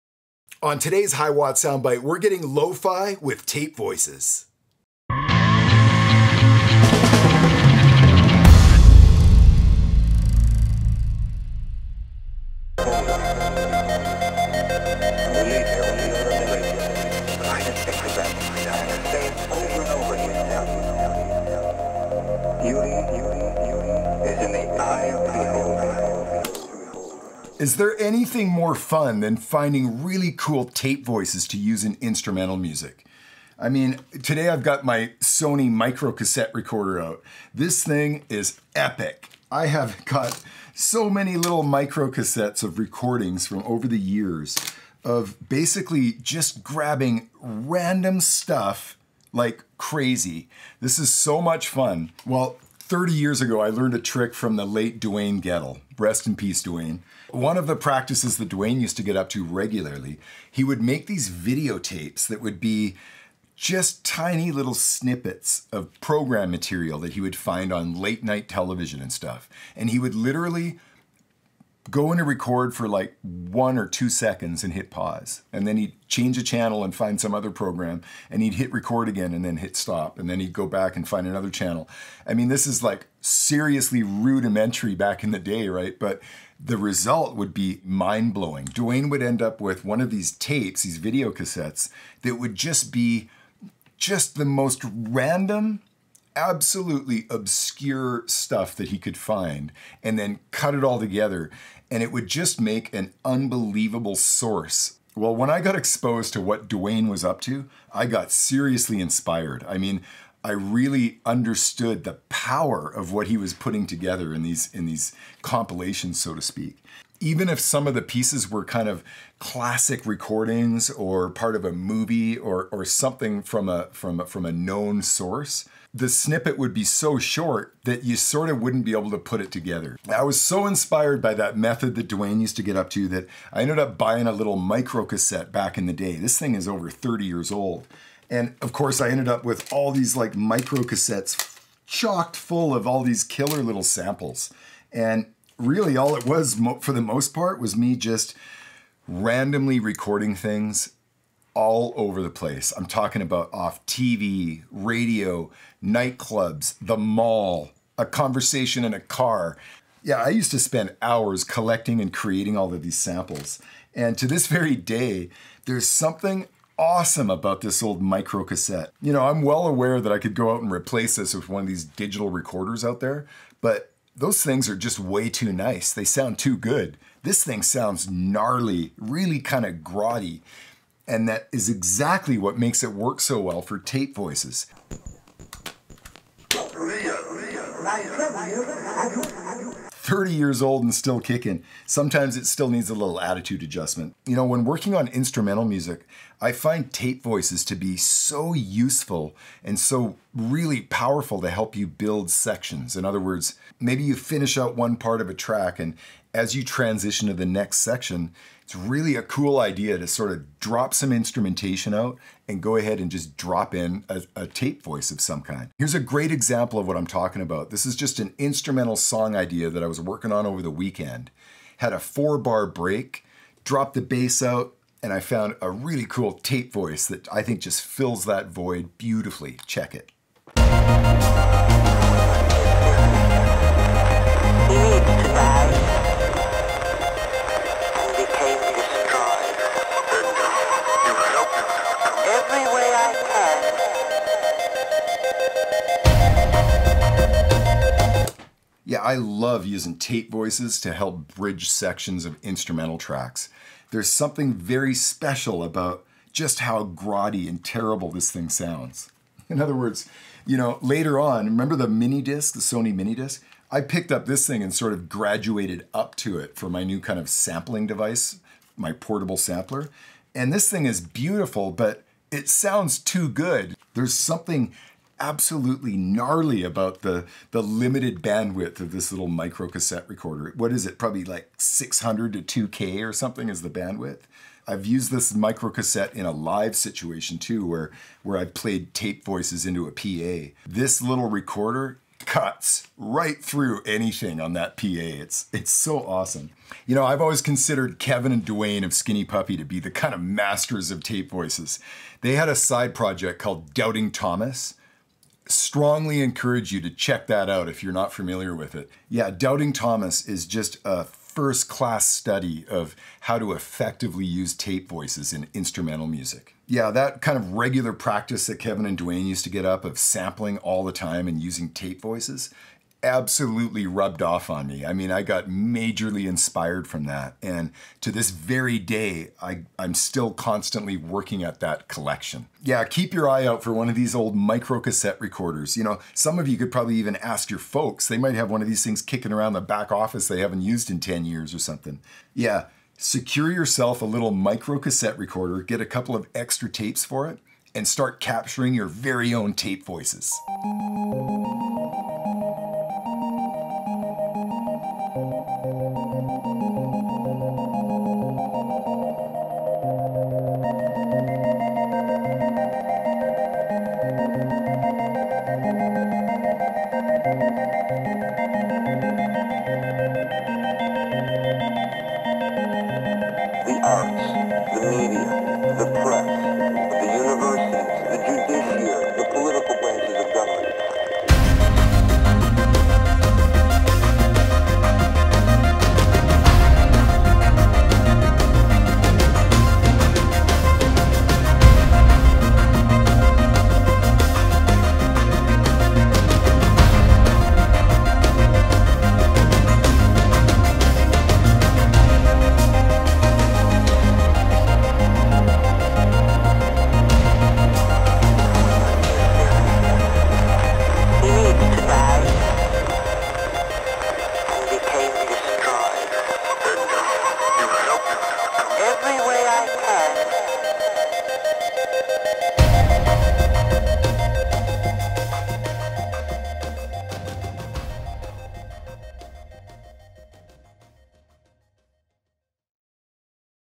Tape Voices - Producing Instrumental Music
In today's session, Ken 'hiwatt' Marshall is playing TAPE VOICES!!!  Spoken word samples are a powerful resource for instrumental music!  Sit in on today's session and hear a couple examples ;-)